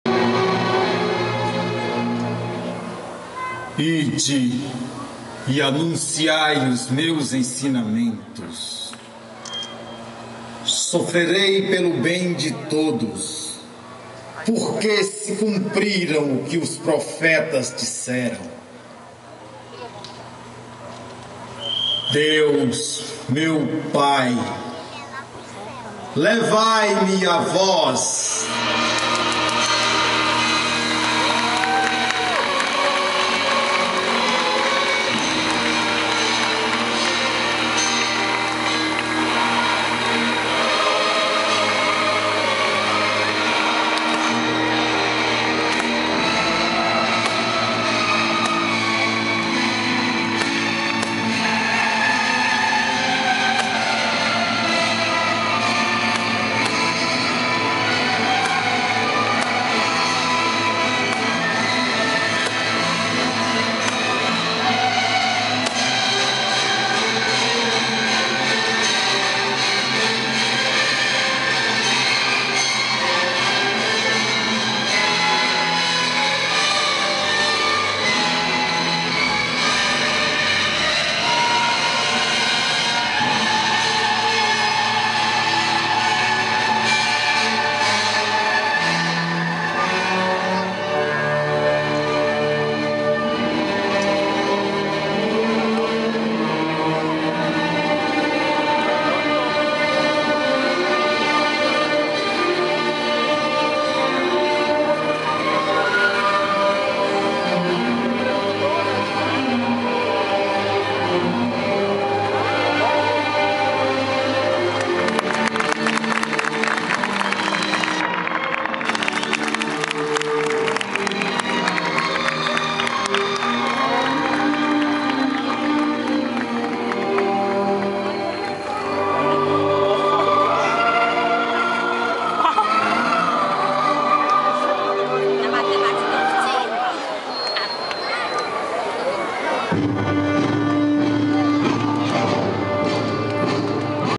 PAIXAO DE CRISTO FORTALEZA 2013 - ENCERRAMENTO JESUS SUBINDO AO CEU
Espetáculo exibido no Centro Cultural Dragão do Mar na Semana Santa de 2013